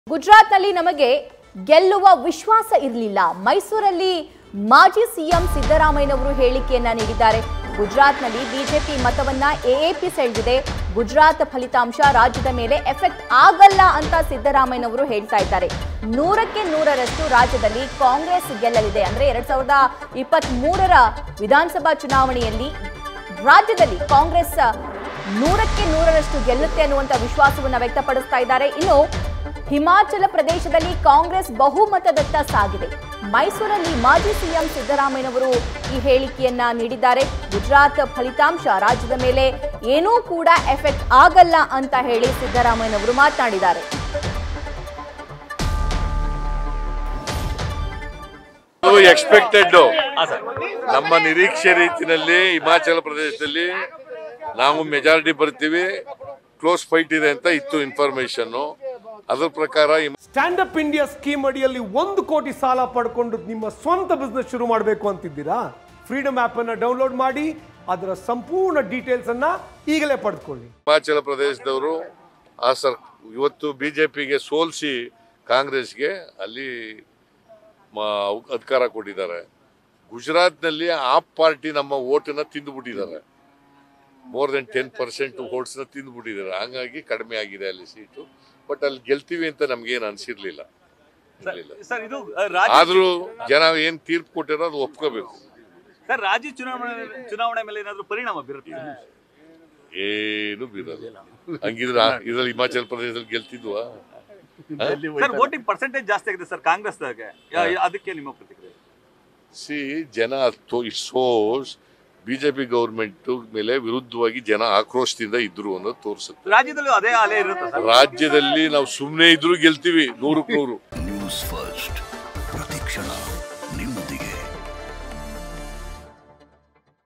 Siddaramaiah : ಗುಜರಾತ್​ನಲ್ಲಿ ನಮಗೆ ಗೆಲ್ಲುವ ವಿಶ್ವಾಸ ಇರಲಿಲ್ಲ.. | Gujarat Election Result 2022
Siddaramaiah : ಗುಜರಾತ್​ನಲ್ಲಿ ನಮಗೆ ಗೆಲ್ಲುವ ವಿಶ್ವಾಸ ಇರಲಿಲ್ಲ.. | Gujarat Election Result 2022 | @newsfirstkannada